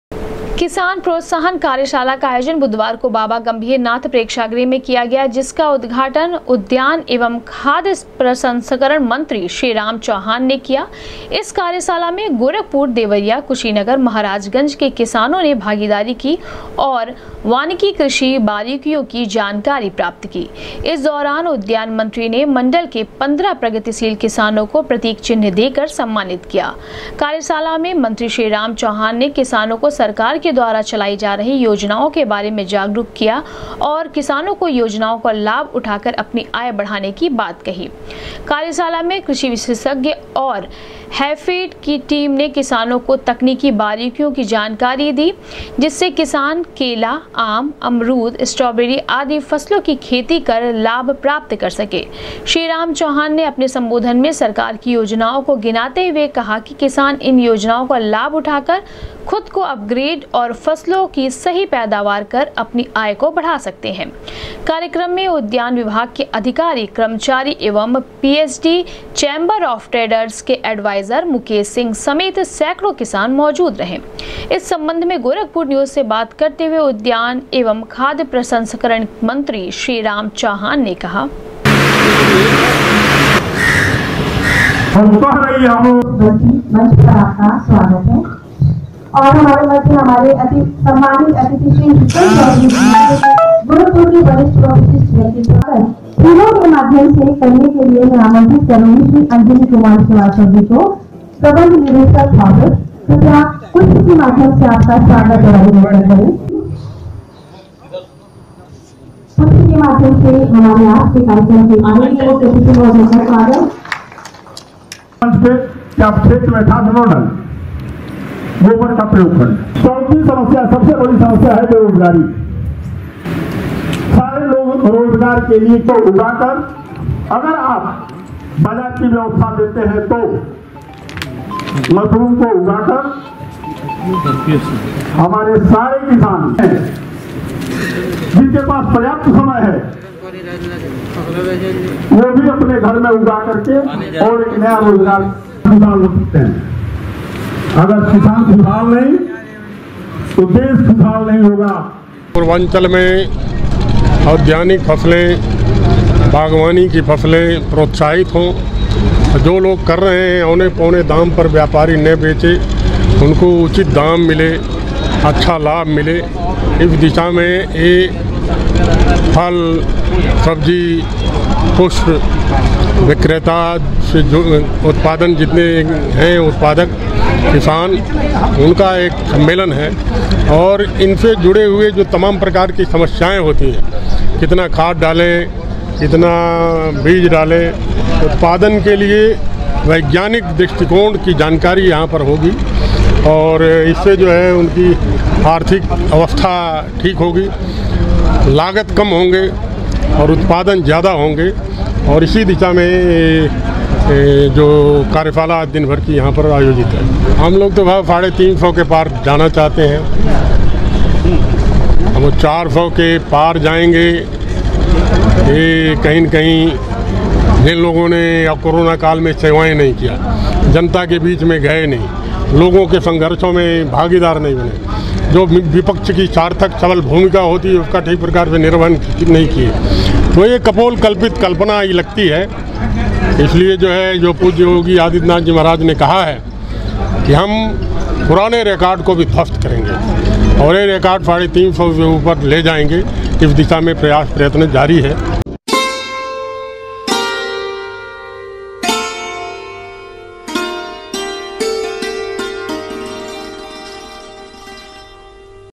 बागवानी फसलों की जागरुकता के लिए किसान प्रोत्साहन कार्यशाला का किया गया आयोजन,किसानों को दी गयी
बागवानी फसलों की जागरुकता के लिए किसान प्रोत्साहन कार्यशाला का किया गया आयोजन,किसानों को दी गयी तकनीकी खेती की जानकारी,उद्यान मन्त्री ने कहा, शासन की योजनाओ से किसान बढ़ा सकते है अपनी आमदनी